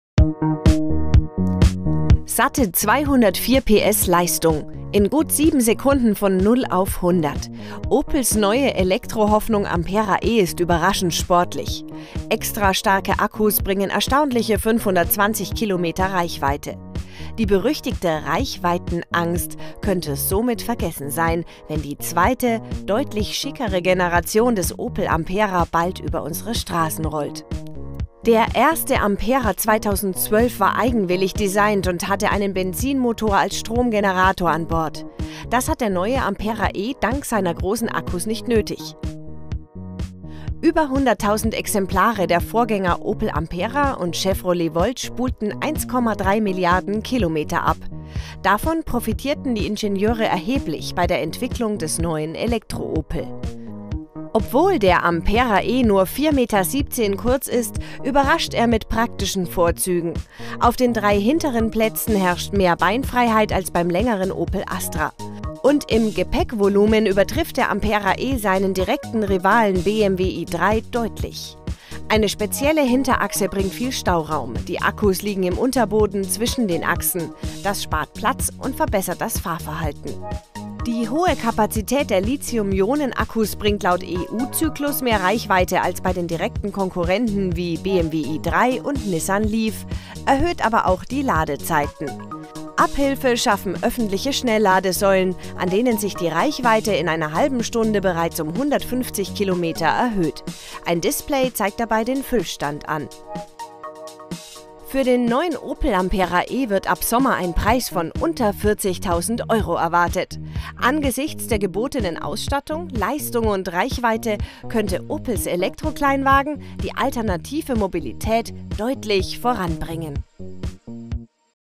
Endlich: Opel zieht allen davon !
Lange hats gedauert aber nun zieht Opel allen anderen Spritfressern davon, der neue Opel Ampera-e macht jetzt tatsächlich eine Reichweite von über 500 Kilometern, hier der Check. (Bild: Opel)